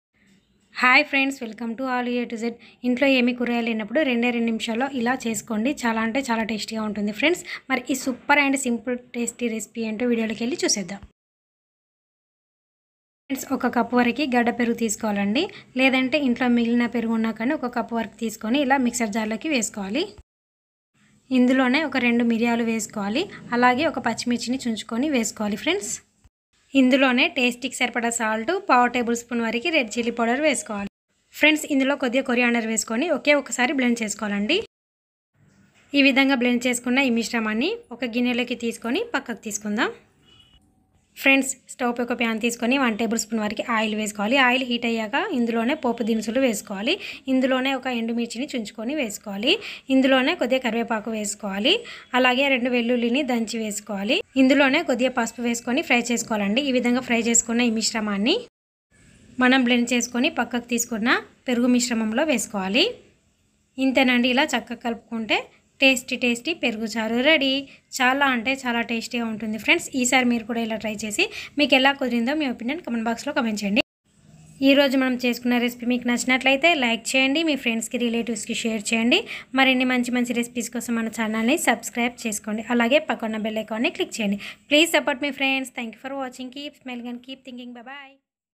బాగా ఆకలిగా ఉన్నప్పుడు ఇంట్లో కూరగాయలు ఎం లేనప్పుడు2ని"ల్లో ఇలాచేస్తే కడుపునిండుతుంది🙏
Easy&Quick Recipe
Alu A-z